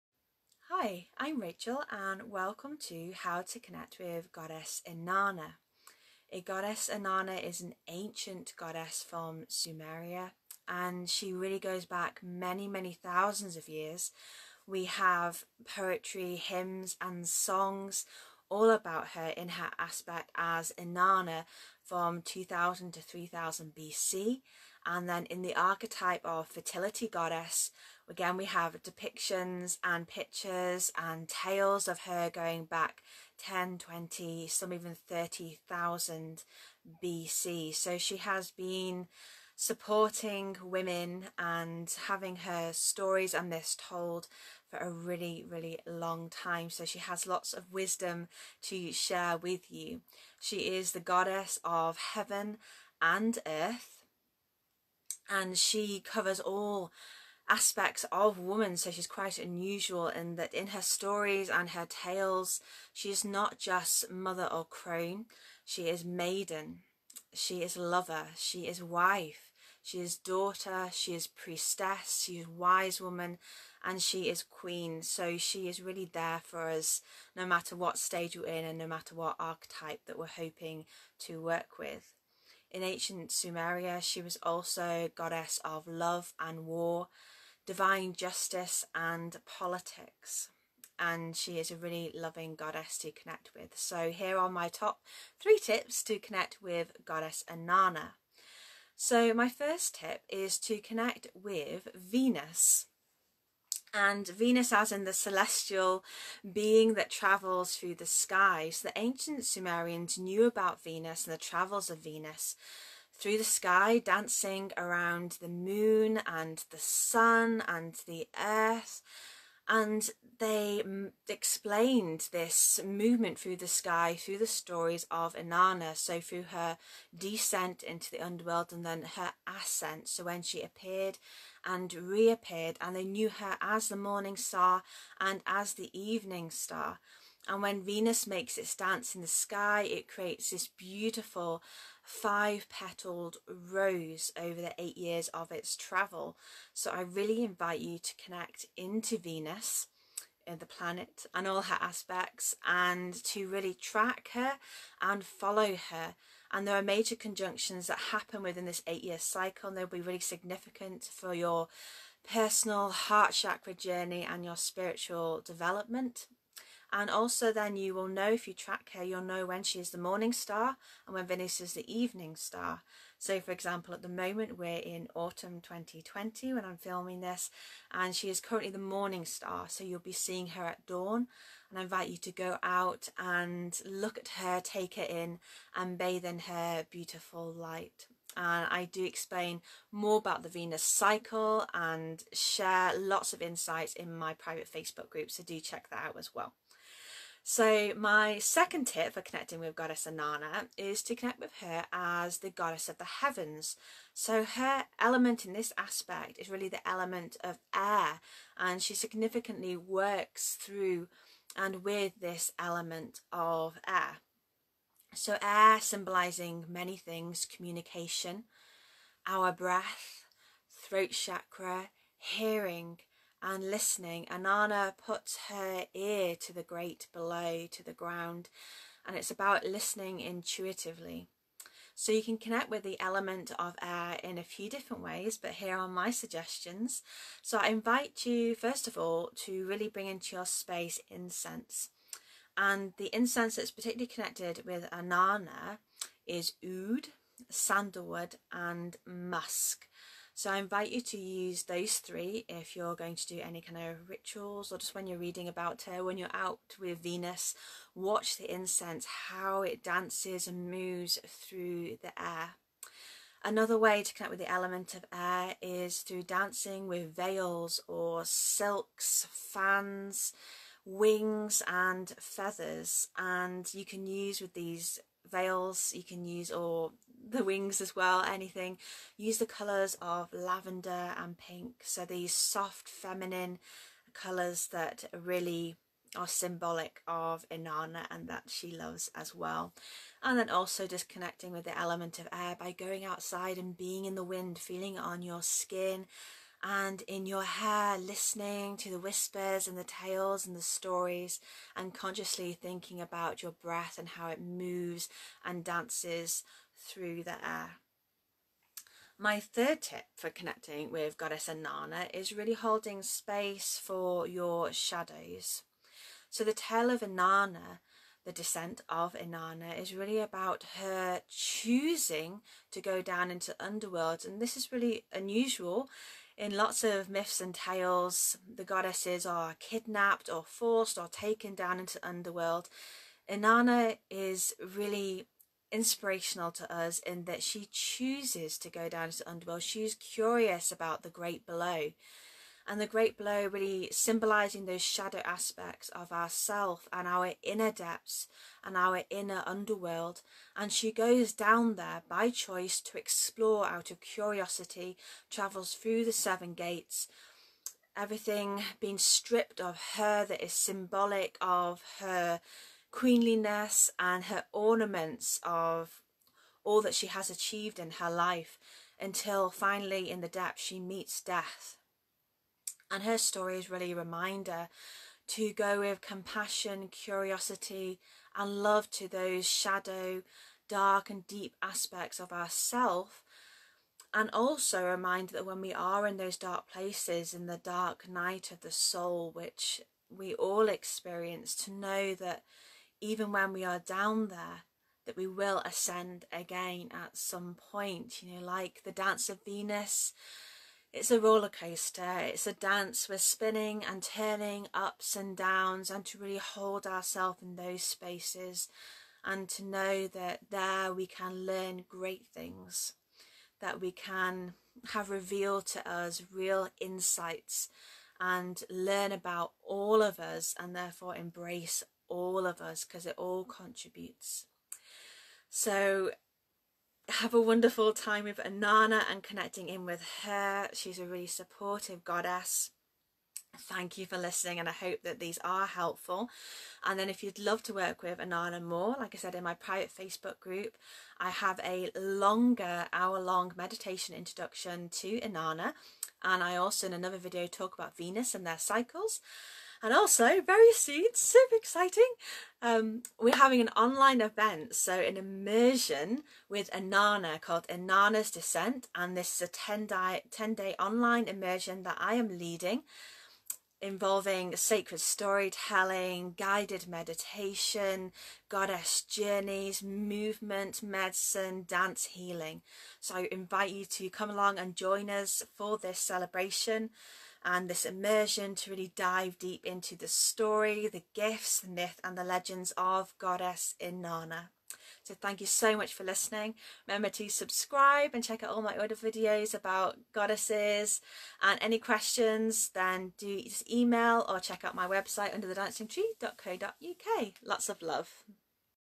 How to Connect with Goddess Inanna
Inanna is an Ancient Sumerian Goddess. In this video learn more about how to connect with her and practices to deepen your relationship with the Goddess of Heaven and Earth.

Enjoy my top 3 tips on how to connect to Goddess Inanna!

📚 Rachel is a best-selling author and internationally renowned author on Goddess Mythology, you can find out more about and purchase her books here: